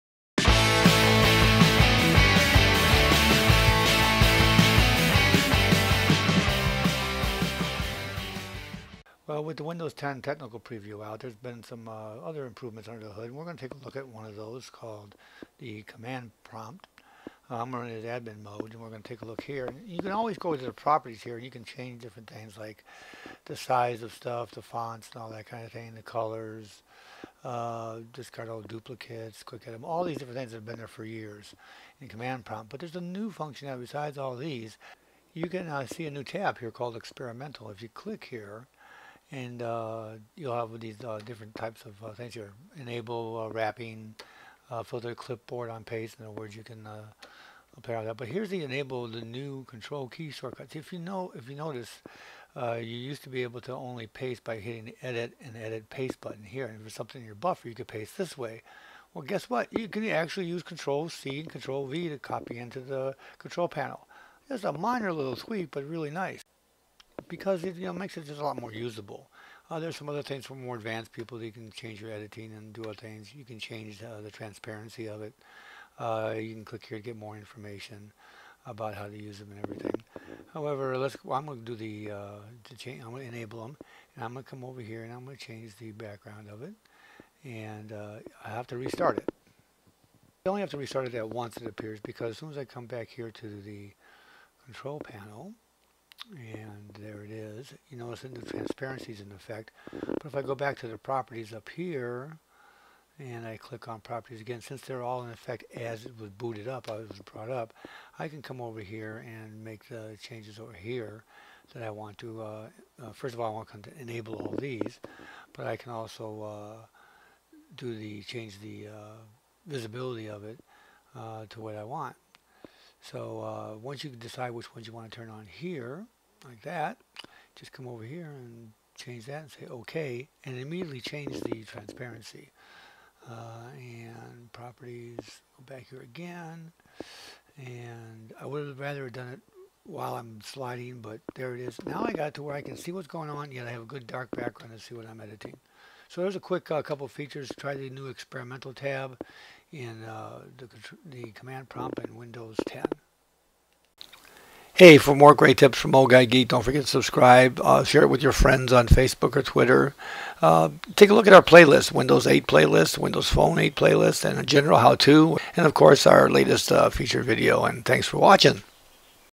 Windows 10 Tips & Tricks - Command Prompt Experimental Settings
Well, it's about time they paid attention to the Command Prompt.  There are some new features in it that are long overdue.

There are 2 in particular that will help you use it more efficiently. Copy and pasting and transparency.